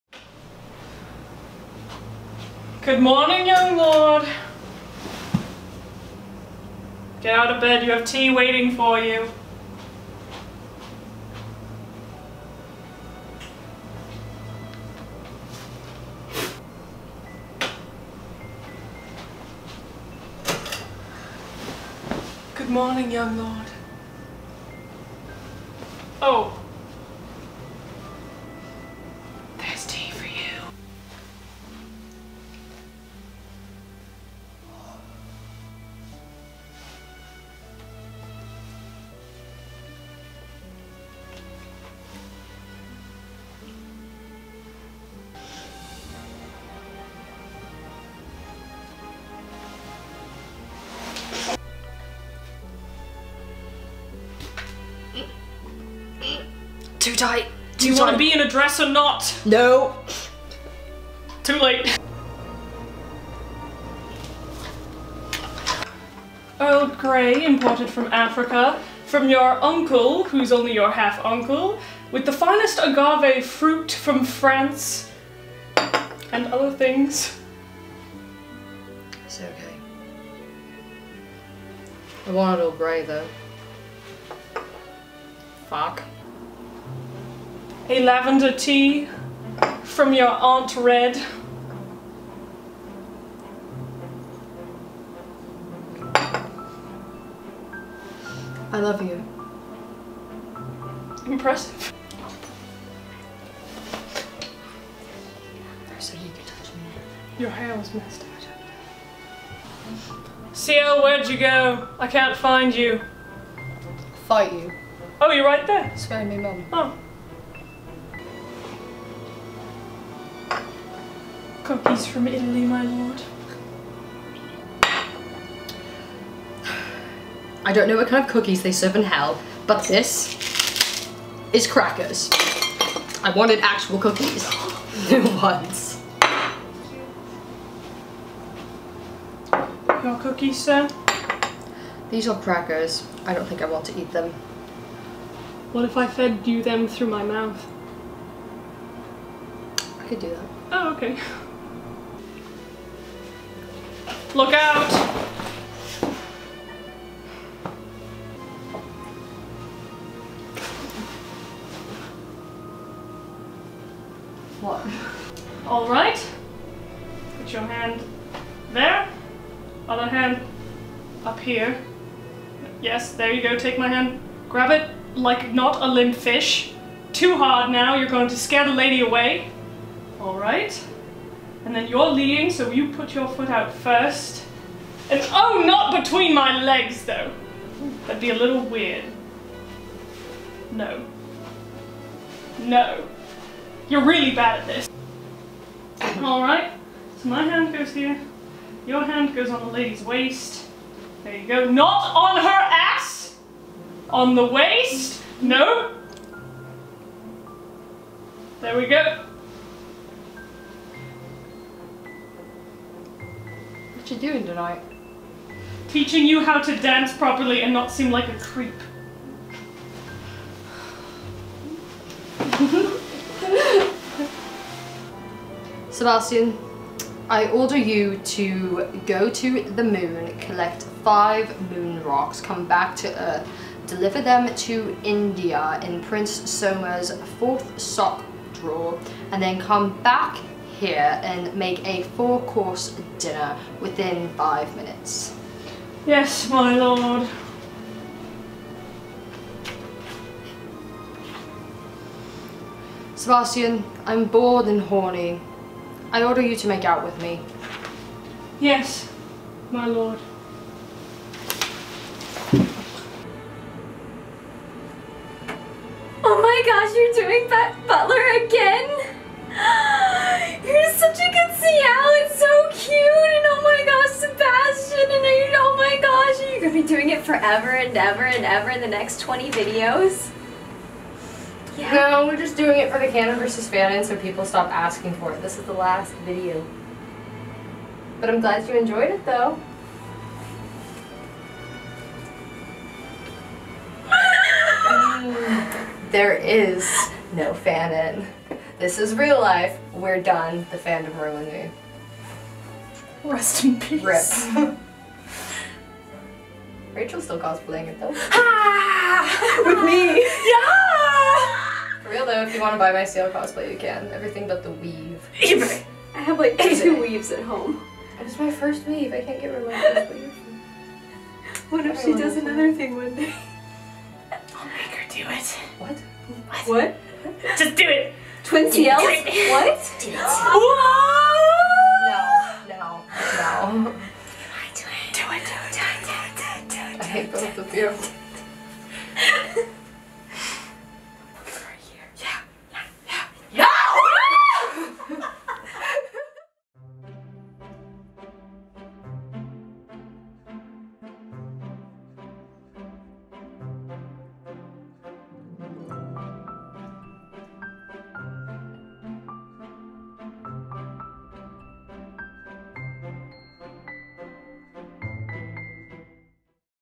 CANON vs FANON SebaCiel | Black Butler
Everyone's favorite series, Canon vs Fanon, this round is SebaCiel from Black Butler, the series that will probably forever plague my miserable existence.

Man it was weird cosplaying Ciel again.

(This is a Super Old Video, But If You Liked It, Check out Some Newer & Better Stuff on the Channel!!!)

CAST + CREW
Cinematographer | @kennyletelier1458
Sebastian Michaelis | @Svattii
Ciel Phantomhive | Mishkali

♡SOCIAL MEDIA♡